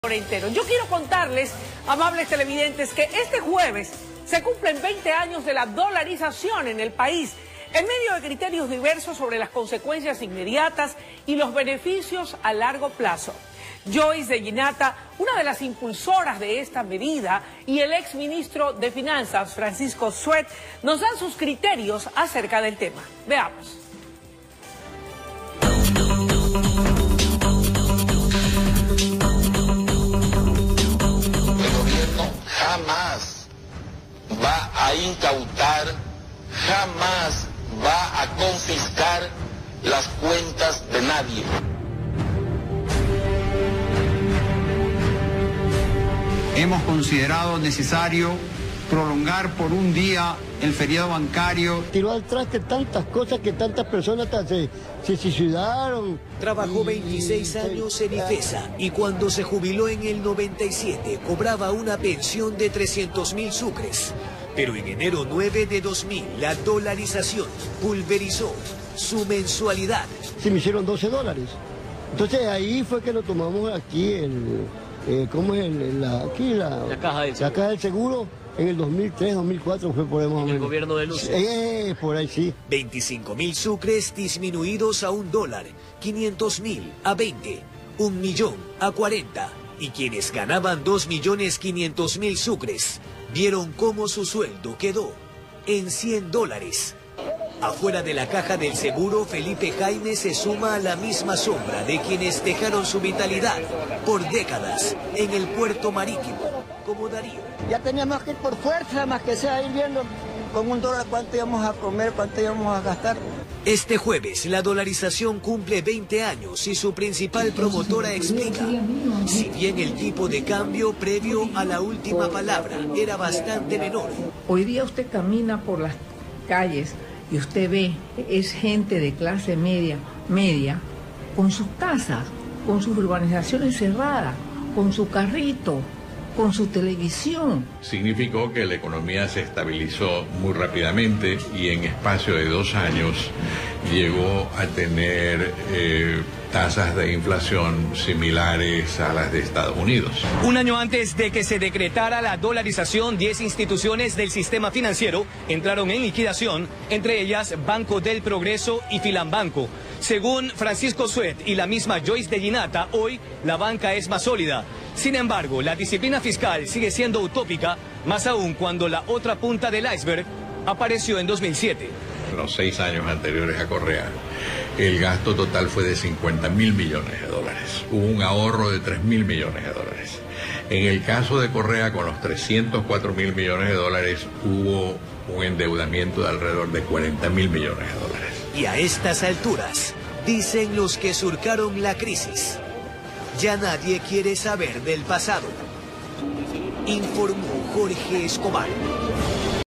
Joyce de Ginatta hace un balance a veinte años de la dolarización
La impulsora de esta medida económica nos da sus impresiones luego de dos décadas

Recordemos que el impacto de este cambio afectó a gran parte de la población, quienes sintieron que sus sueldos y pensiones se degradaban.